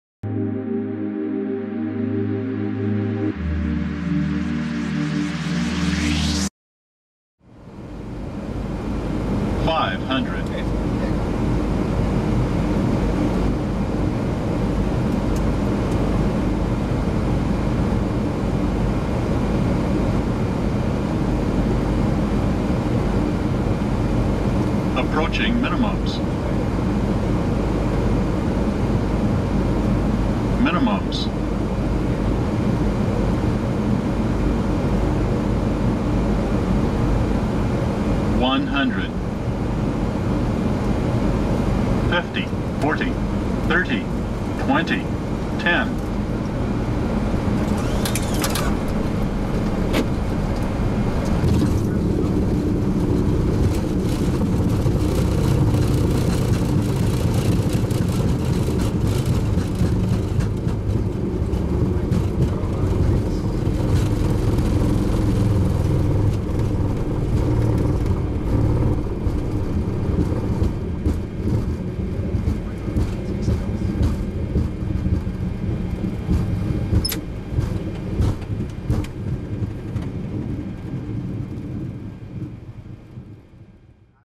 COCKPIT - Boeing 737 Pilot Throttle Inputs during Landing
Have you ever wanted a SCALE MODEL of a PLANE you've SEEN or FLOWN? Search no more, go to:

Hello everyone and welcome to this special PlanesWeekly video.

Over the last couple of years we have had the great pleasure of being in the jumpseat several times and for this video we thought about being a little more creative with it. Watch as the captain of a Boeing 737-800 almost instinctively throttles up and down to keep the airplane on the correct speed and glide slope for landing. After touchdown, the speedbrakes deploy and the captain applies reverse thrust to slow the airplane. The first officer then calls out "60 knots" and the captains shuts down the reverse thrust.

Equipment details can be found on our channel.